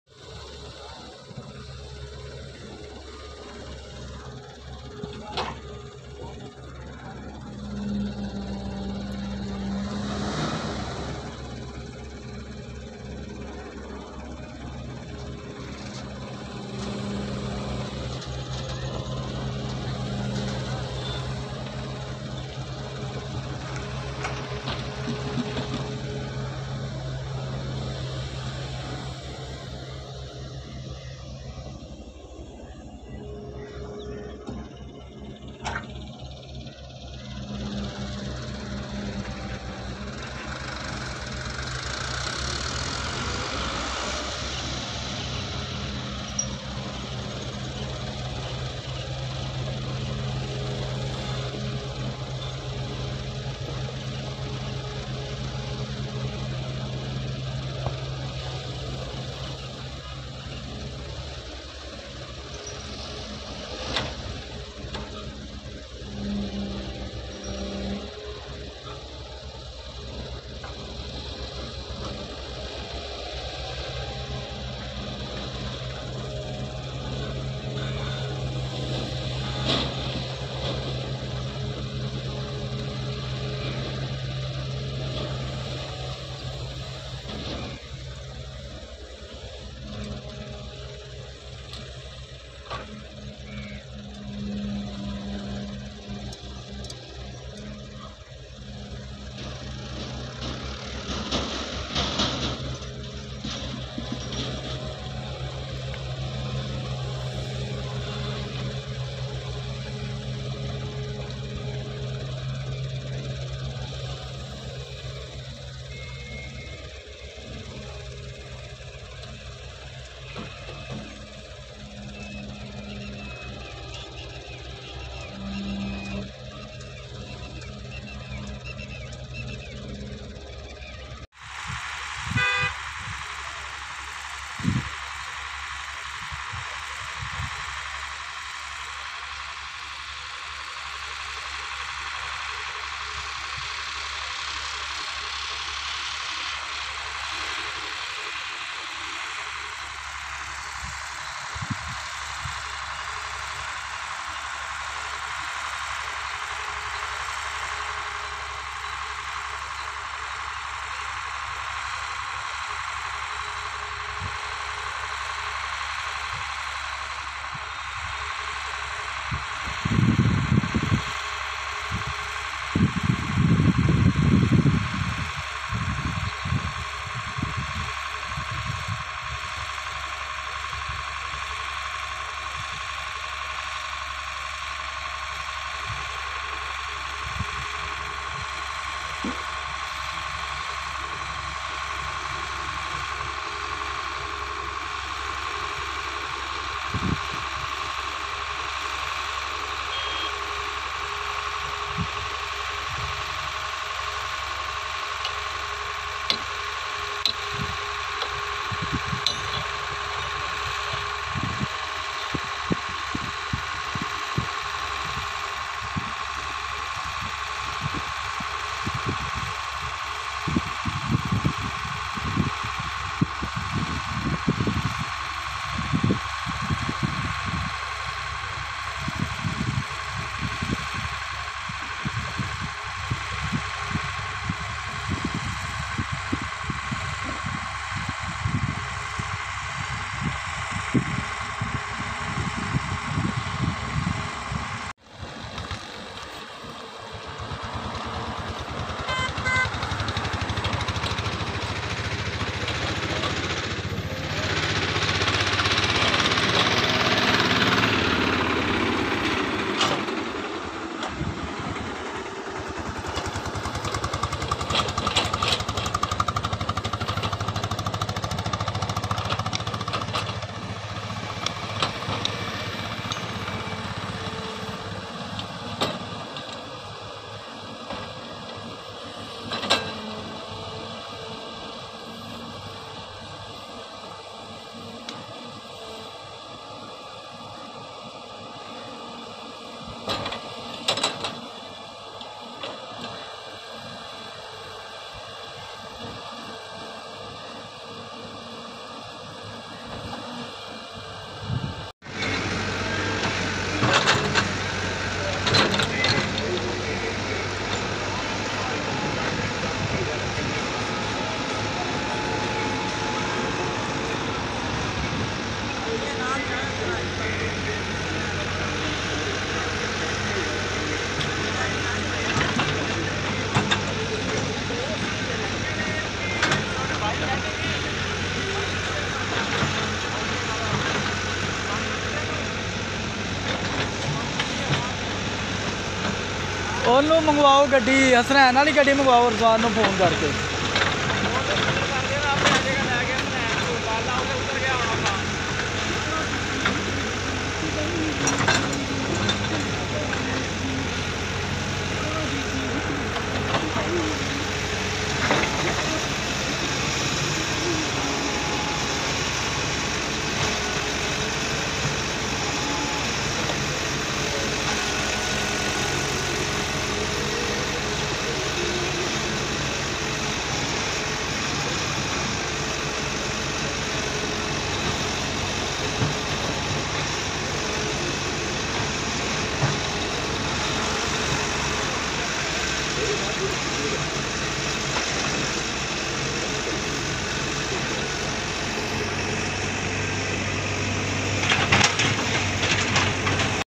Different activities for construction worke related with different heavy-duty machinery at site
Different activities for construction worke related with different heavy-duty machinery at site for new construction